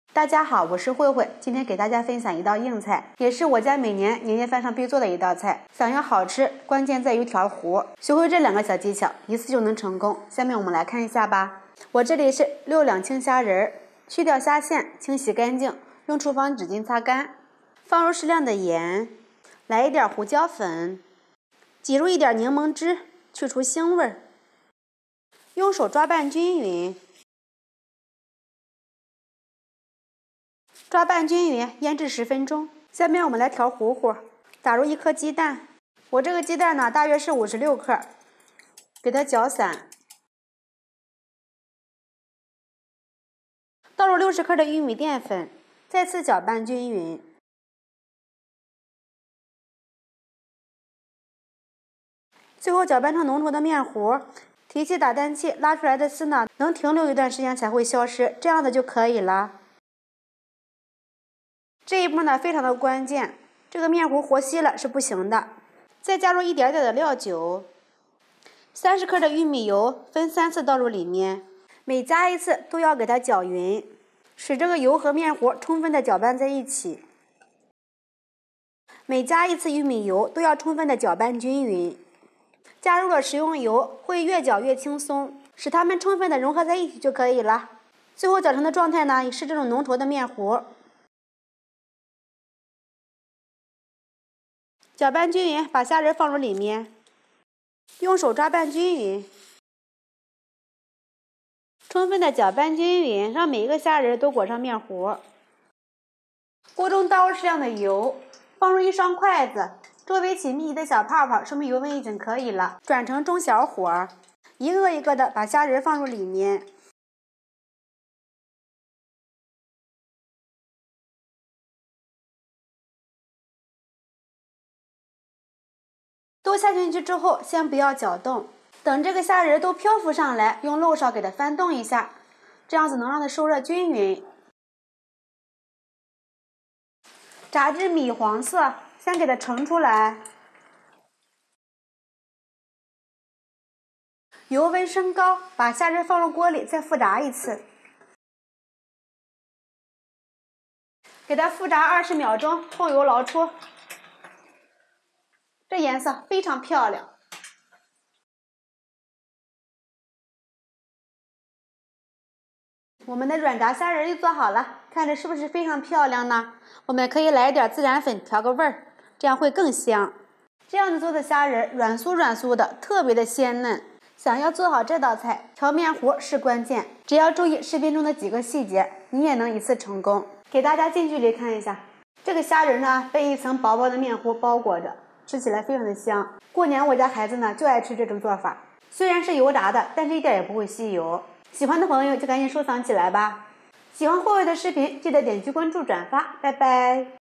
软炸虾仁的正确做法，外酥里嫩好吃不腻，待客必做的一道拿手菜【食在小厨】
软炸虾仁的家常做法，色泽金黄，口感酥脆，吃出满满的高级感
"我们的视频着重展示美食制作过程，所有食材的来源都符合合法和道德标准。"